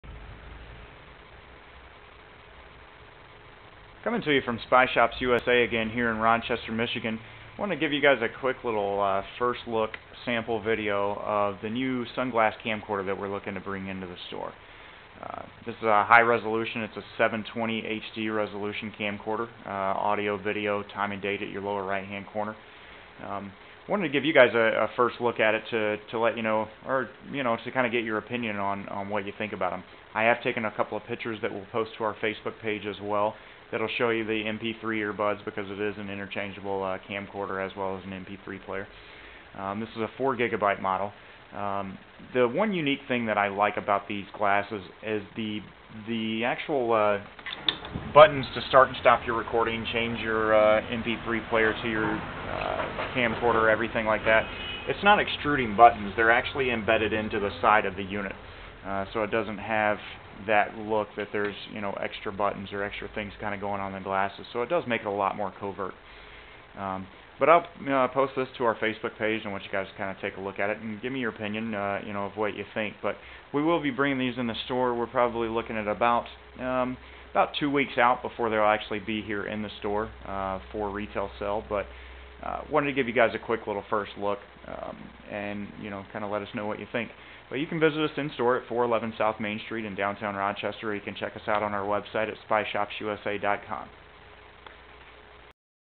New HD Sunglasses DVR
This is a new product that we are testing and wanted to get your opinion on it. We will most likely be bringing these in the store toward the end of the month. Let us know if you have any questions or concerns.